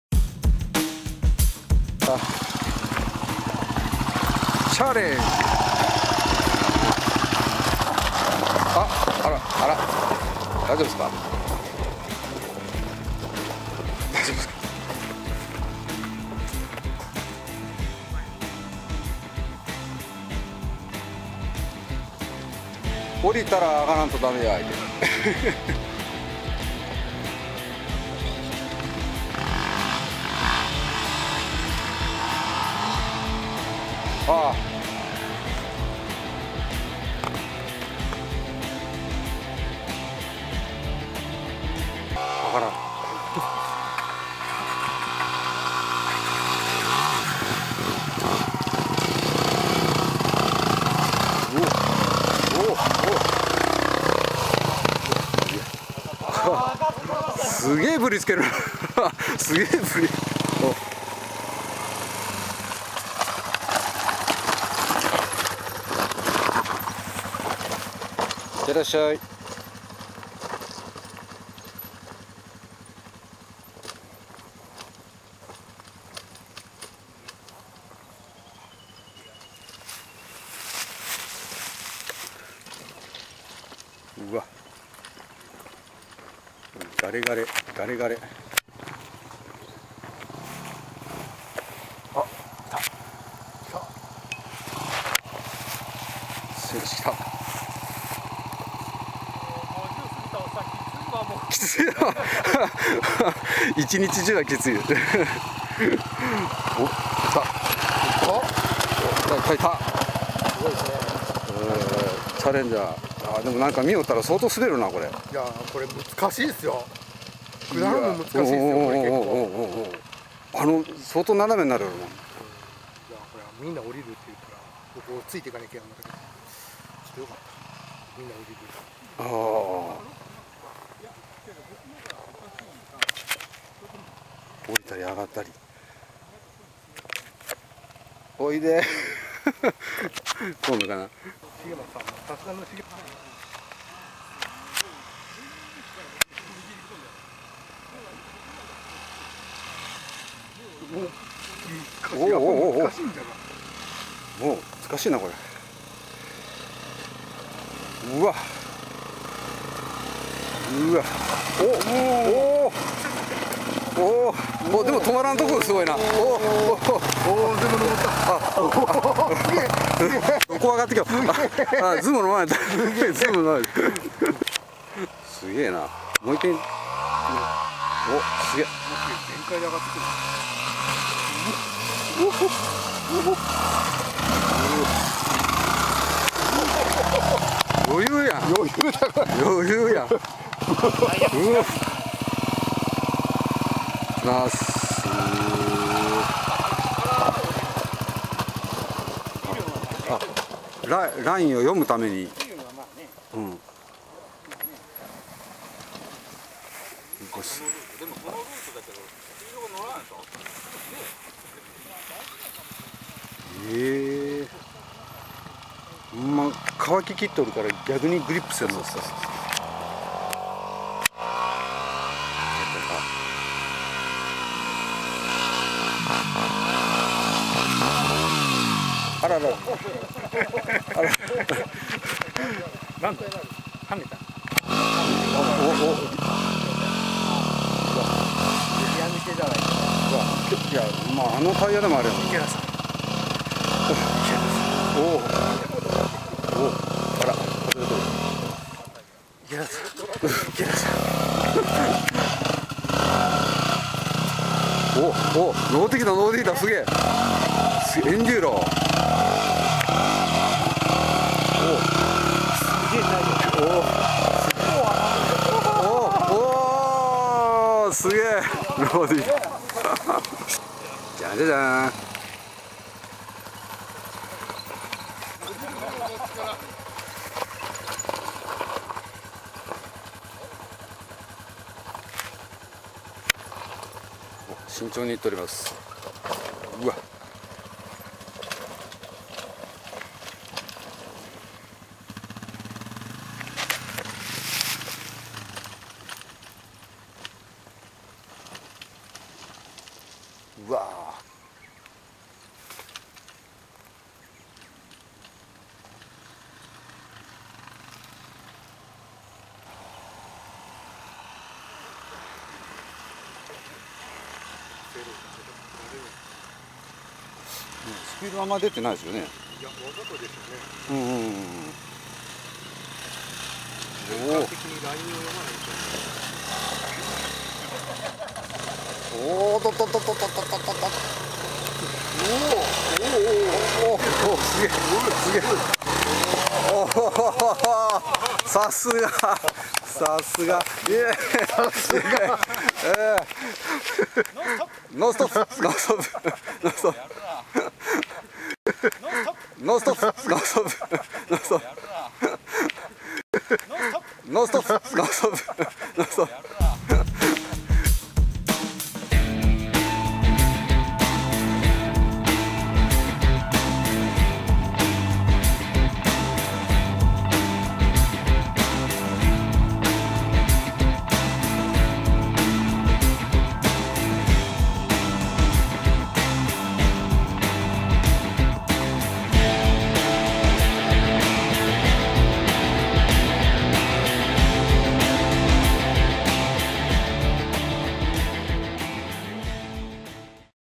四駆広場で遊ぶ♪⑬【結界坂で遊ぶ!!】
テラノ坂の何個か手前の坂が結界坂と言うらしい…その坂で変態ライダーが遊んでいるシーンです!!　見てね♪

このシリーズはコチラからご覧いただけます。